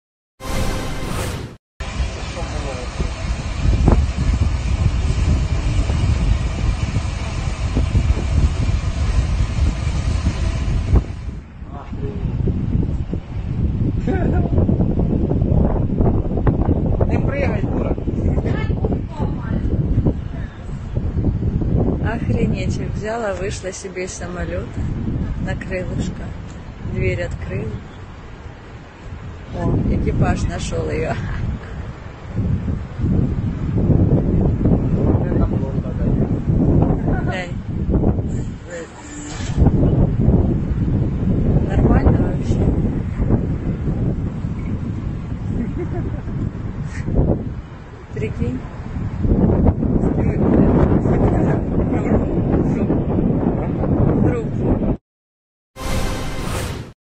Борисполе женщина вылезла на крыло самолета
Прямо перед взлетом самолета в аэропорту «Борисполь», женщине стало слишком жарко, и она решила выйти прогуляться на крыло самолёта через аварийный выход.